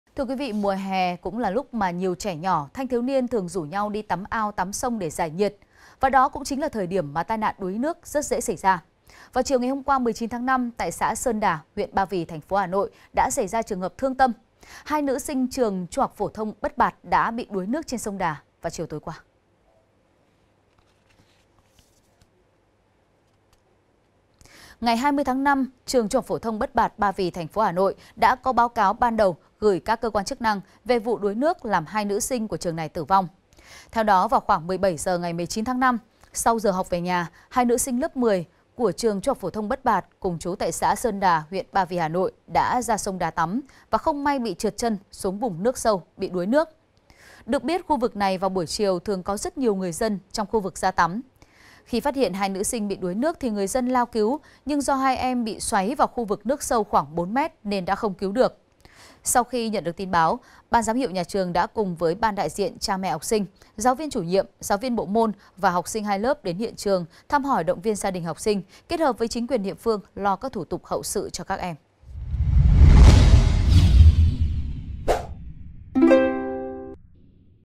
Hà Nội: Hai nữ sinh chết đuối trên sông Đà | VTC14
VTC14 | HÀ NỘI: HAI NỮ SINH CHẾT ĐUỐI TRÊN SÔNG ĐÀ
Mùa hè đến là lúc mà nhiều trẻ nhỏ, thanh niên thường rủ nhau đi tắm ao, tắm sông để giải nhiệt. Và đó cũng chính là thời điểm mà tai nạn đuối nước dễ xảy ra. Chiều qua 19/5, tại xã Sơn Đà huyện Ba Vì TP Hà Nội đã xảy ra trường hợp thương tâm, 2 nữ sinh trường THPT Bất Bạt đã bị đuối nước trên sông Đà vào chiều tối qua.